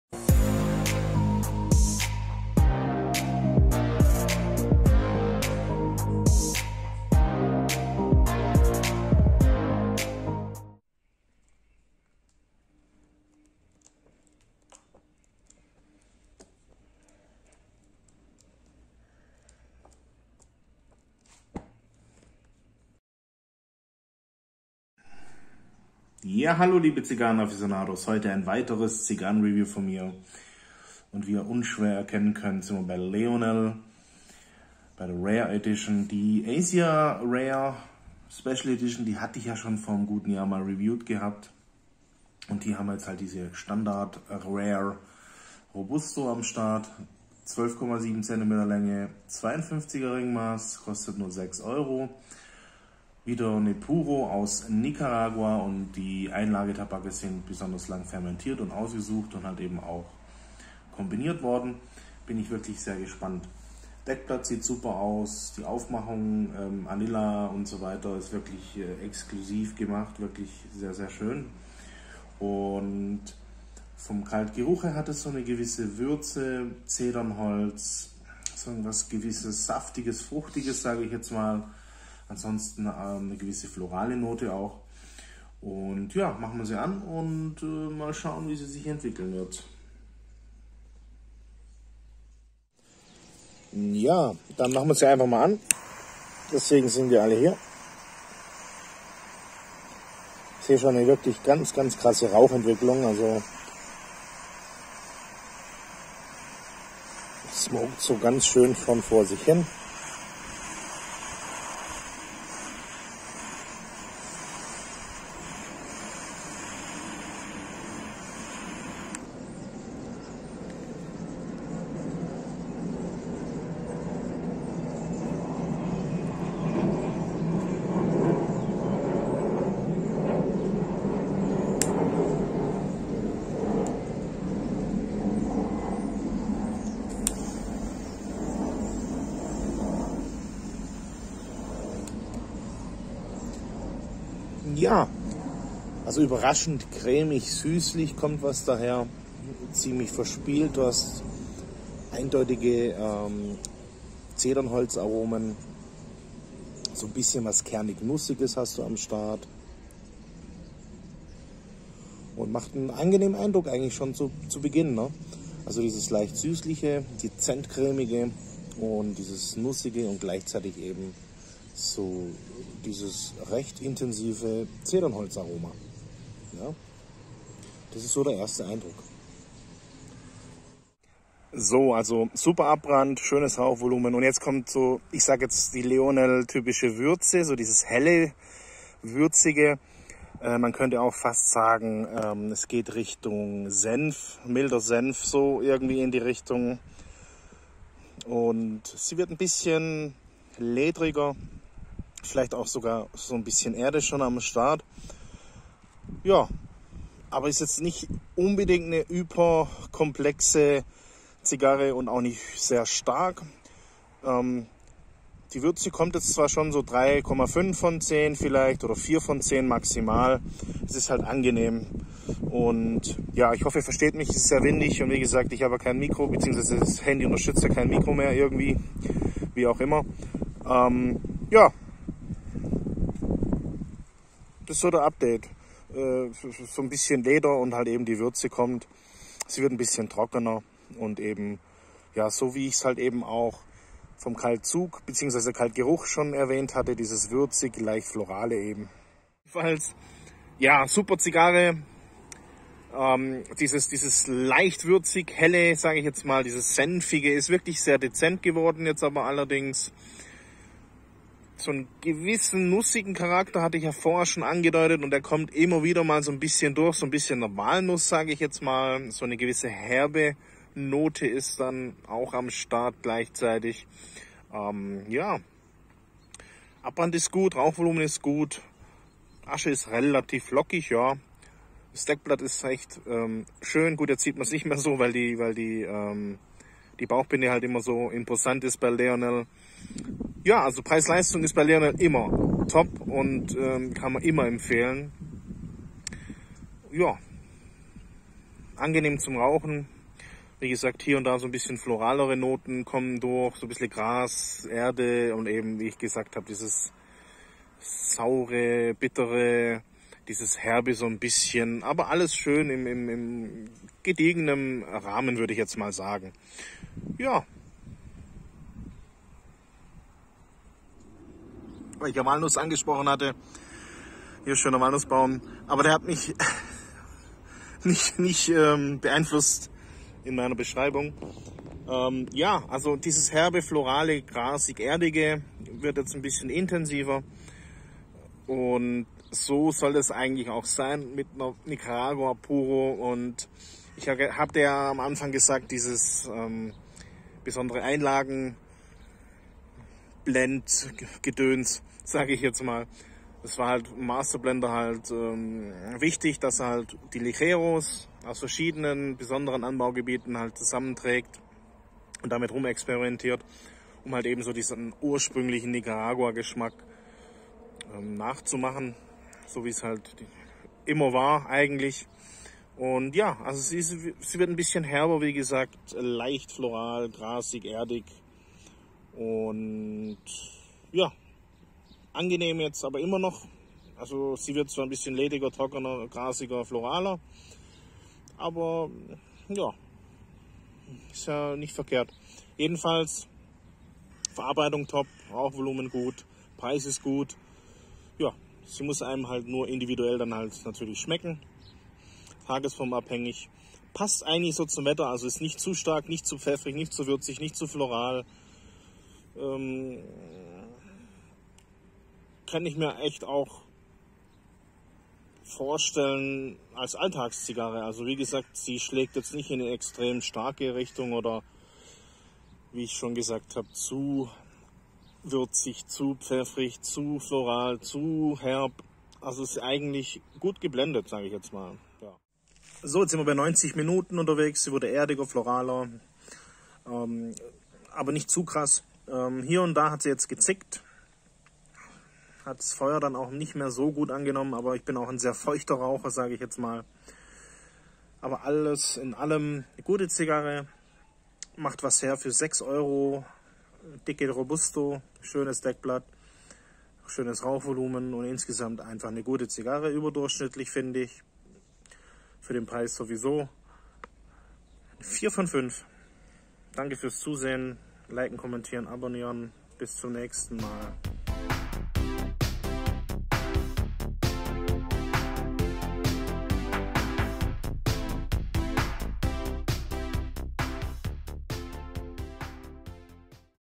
Leonel Rare Robusto | Zigarren Review
und hier eine leckere Nicaragua Puro von Leonel für 6€.

erhältlich im Paul Bugge Store.

Wenn ihr mich unterstützen wollt, wäre ich dankbar